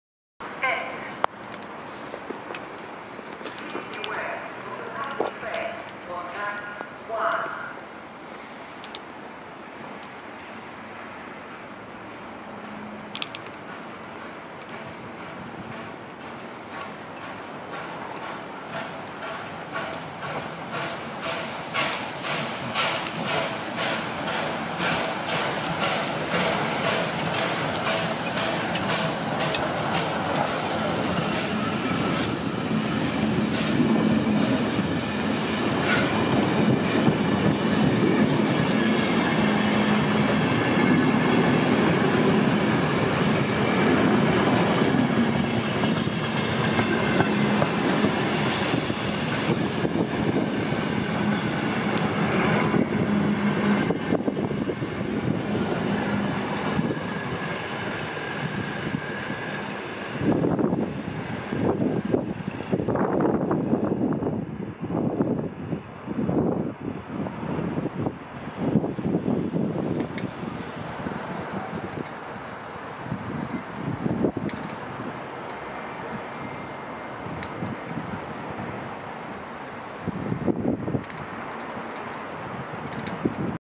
Höyryjuna 1009 ohittaa Kupittaan | Steam train passes Kupittaa, 23.2.2017
Steam train passes Kupittaa station in Turku, Finland. Train was about 2,5 hours late.
Höyryjuna ohittaa Kupittaan aseman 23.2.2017. Juna oli n. 2,5 tuntia myöhässä aikataulusta. Olin jo luovuttamassa kun juna tuli.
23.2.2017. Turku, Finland